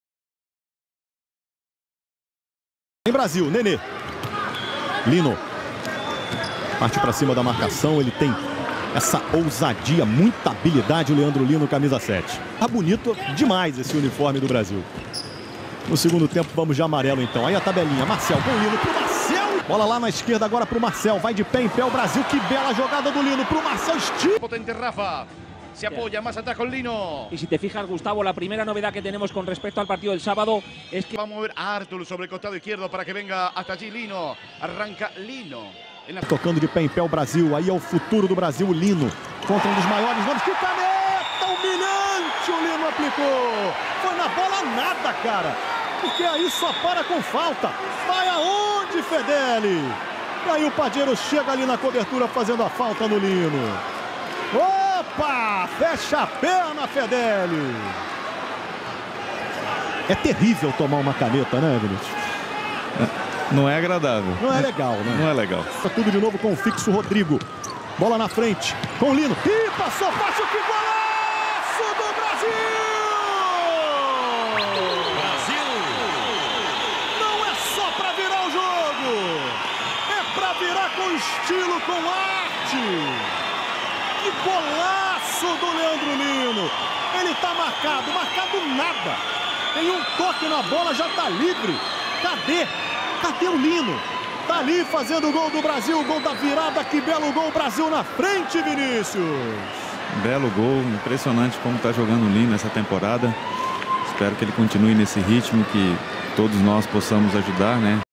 ALA LEANDRO LINO FUTSAL - PULINHO | WW GAMES ⚽️
Leandro Lino dos Santos, o ''Lino'' fazendo seu drible tradicional.